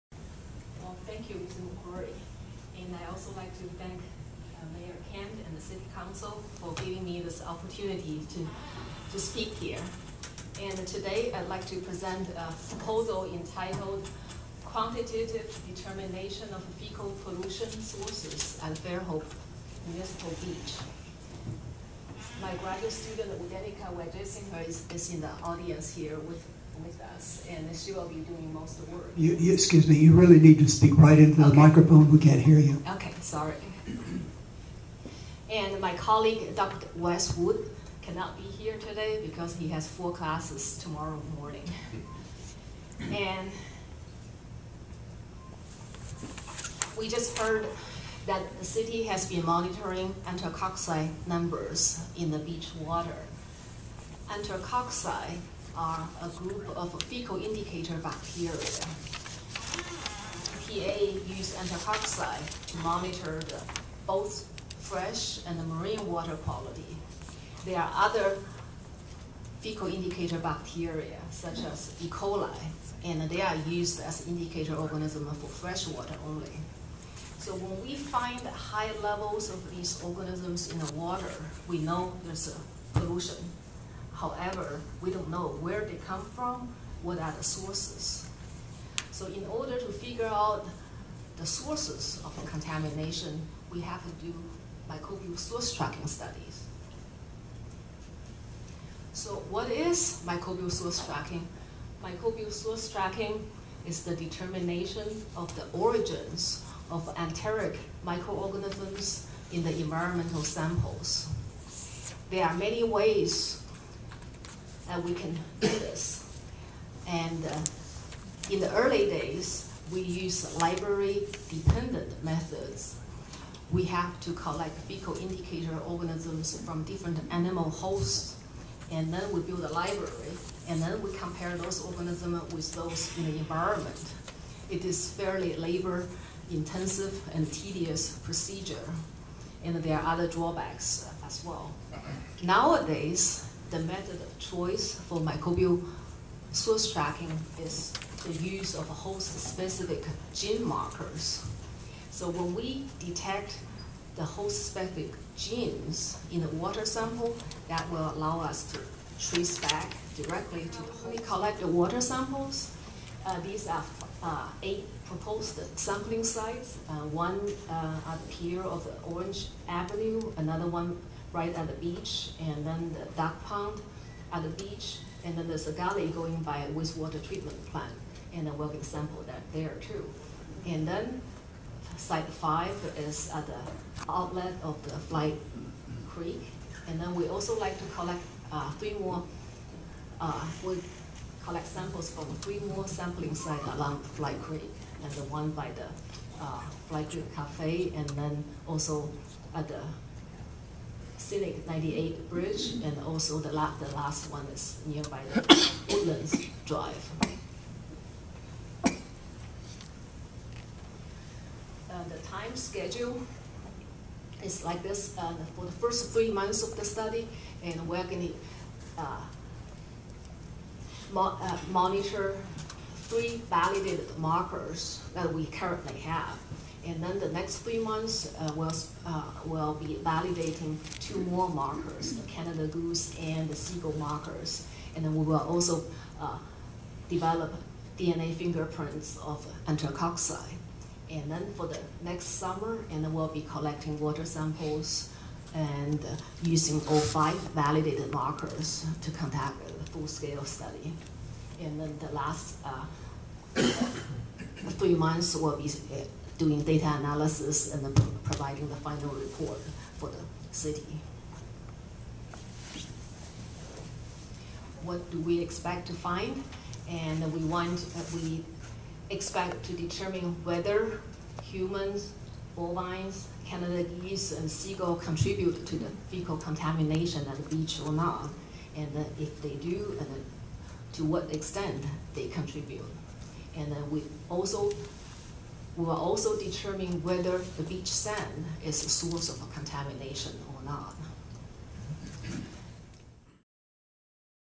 Auburn Professor's Pollution Study
An Auburn University professor may do a study to identify the source of pollution in Mobile Bay near Fairhope.